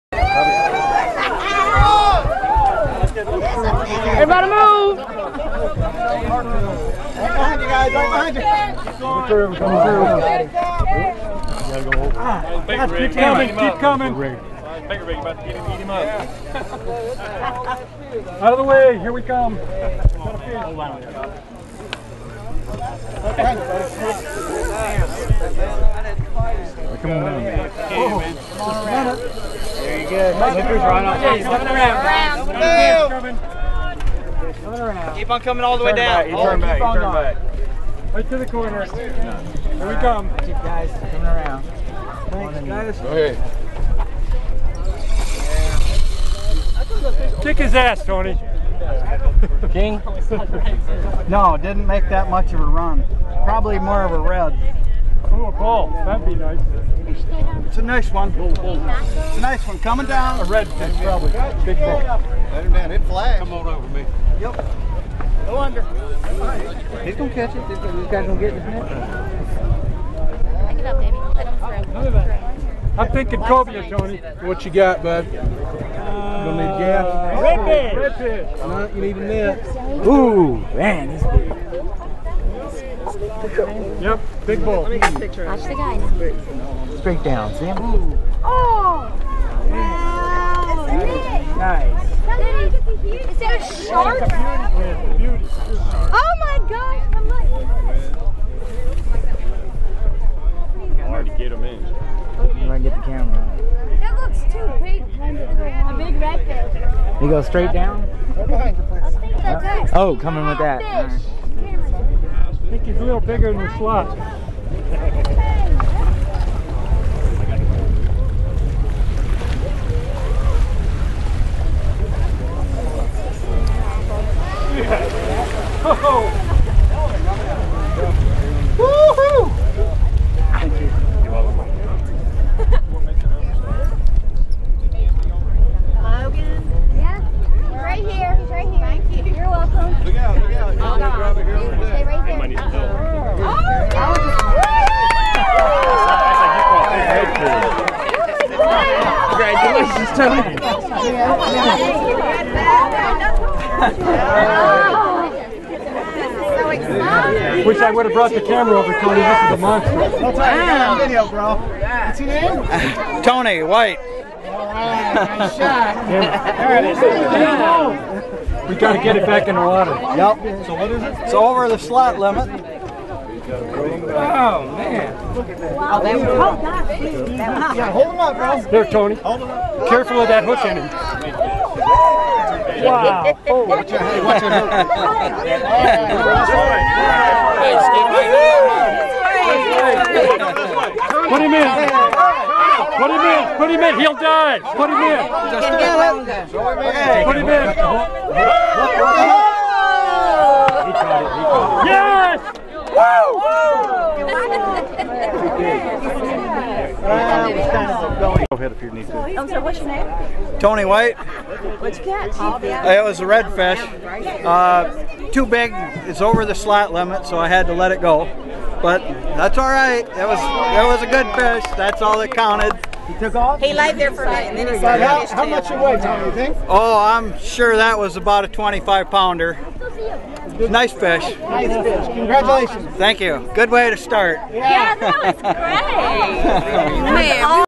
Tony White Catches the First Big One
Tony White catches the first Big Fish off the new Navarre Pier. 25lb of fun and excitement.